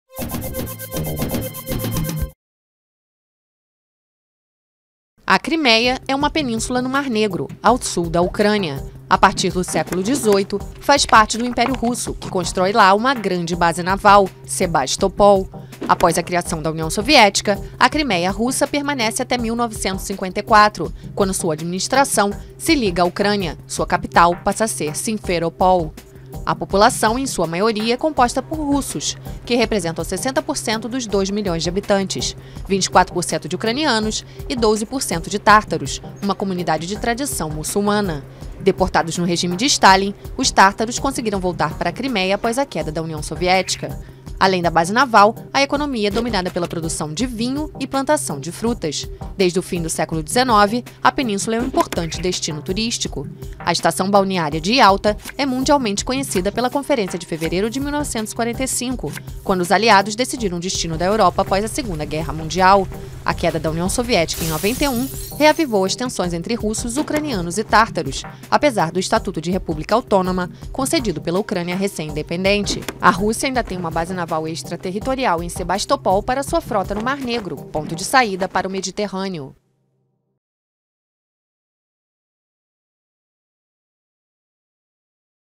A Crimeia
Um ano após um comando pró-russo ocupar o Parlamento da Crimeia, primeiro passo antes de sua incorporação a Moscou, a antiga península ucraniana segue apoiando as autoridades russas, apesar de sua economia abalada, da escassez e de uma inflação galopante. REENVIO